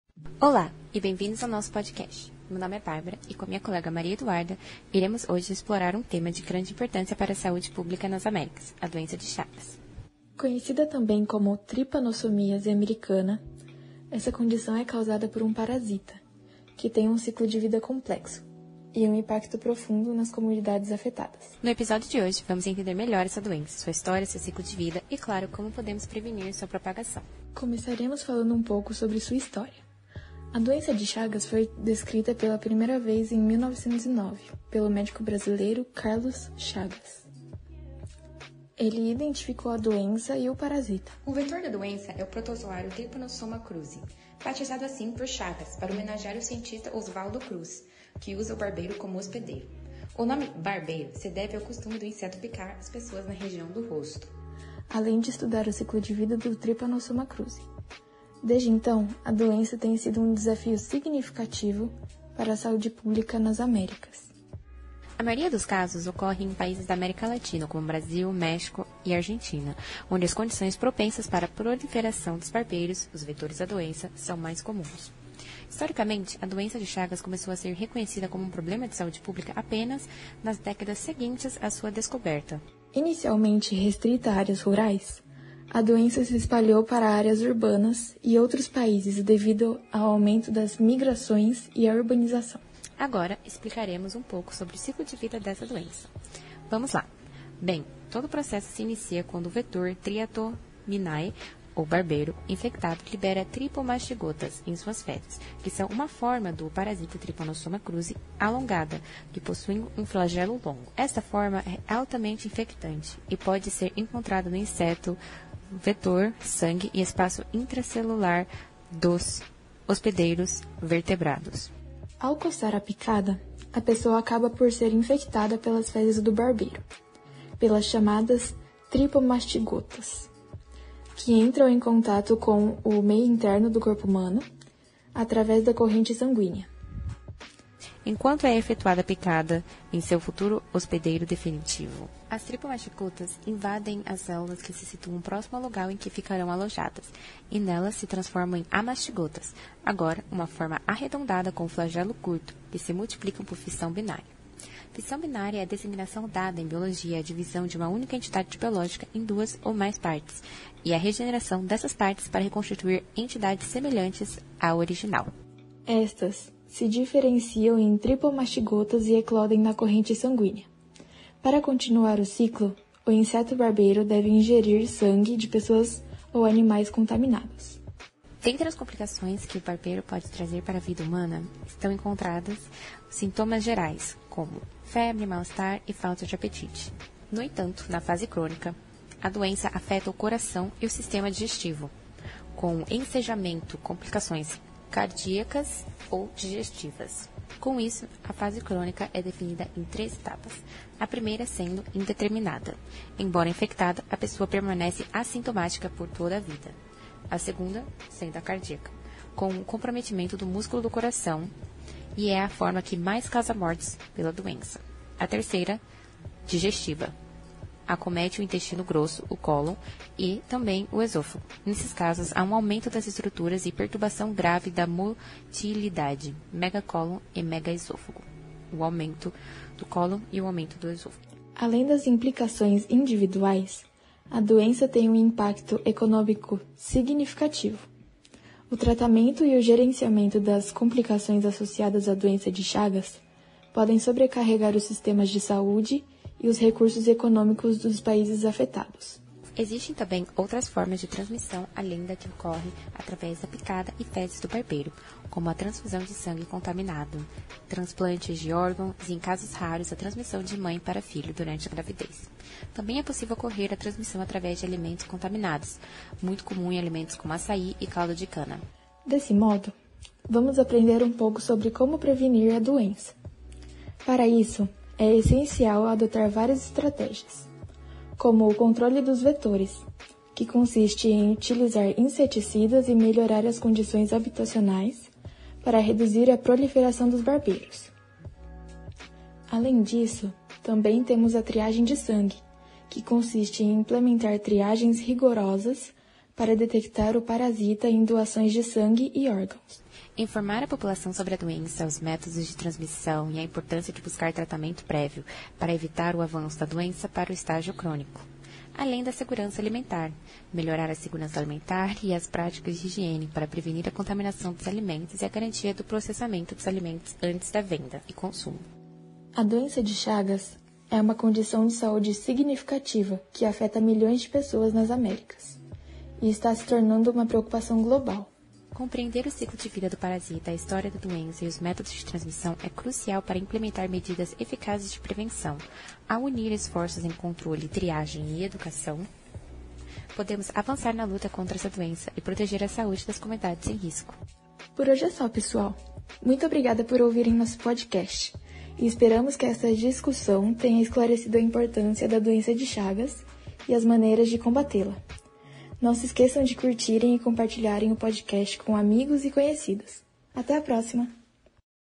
Doença de chagas, conheça sua história e seu ciclo biológico.
Olá sejam bem vindos!, caso tenham interesse venham conhecer um pouco sobre o ciclo de vida, historia e os meios de transmissão da doença de chagas (Tripanosomíasse Americana), causada pelo parasita Trypanossoma Cruzi e transmitida pelo vetor triatominae, o inseto "barbeiro".
Um podcast curto e com imagens ilustrativas.
🔔Ative as notificações para mais conteúdos sobre biologia.
👍 Deixe sua estrelinha  compartilhe e comente sobre o que você achou!